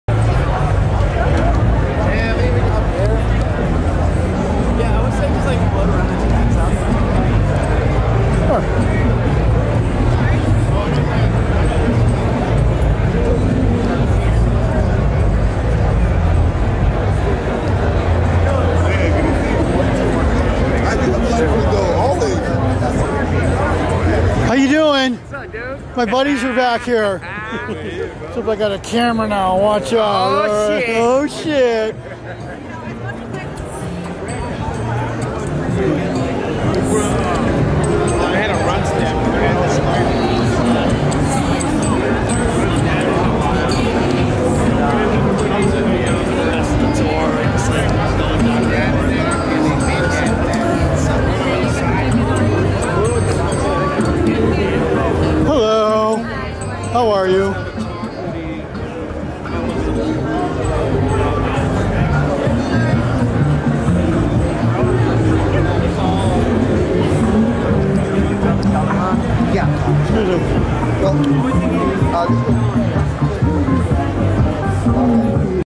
HUMBOLT SEED COMPANY ANOTHER LOOK AROUND THE BOOTH , SANTA ROSA FAIRGROUNDS CA 2021 THE CUP
REALLY NICE BOOTH !!!